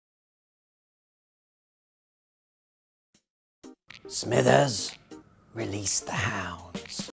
Ted Cruz Release the Hounds
Ted Cruz impersonates Simpson characters - Mr. Burns